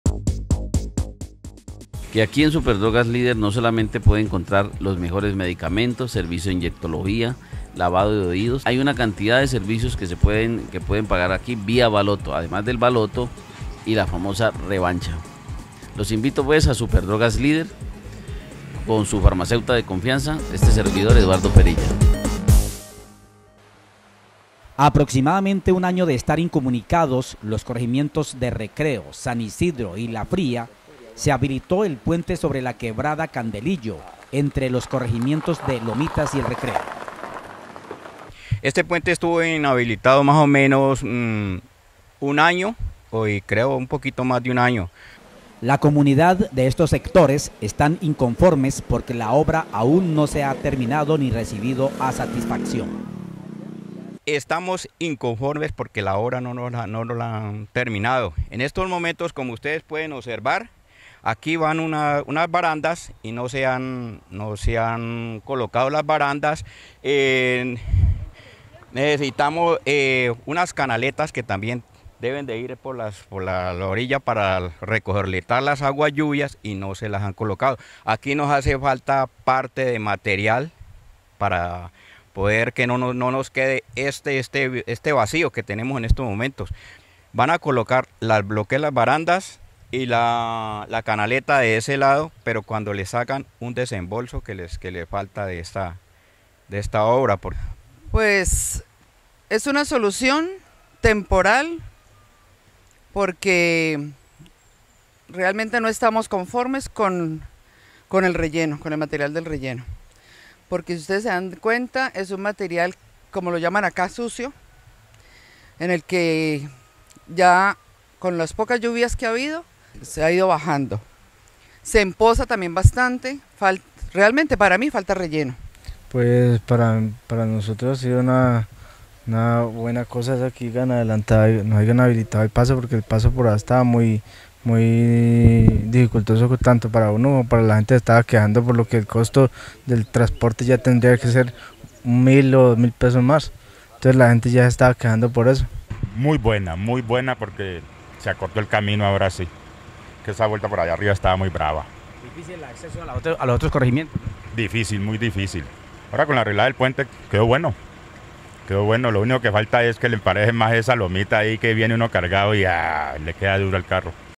Habilitado Puente de Lomitas Recreo
Los habitantes de los corrregimientos de El Recreo  y Lomitas ya pueden comunicarse vía terrestre por el puente de la quebrada Candelillo que estuvo un año fuera de servicio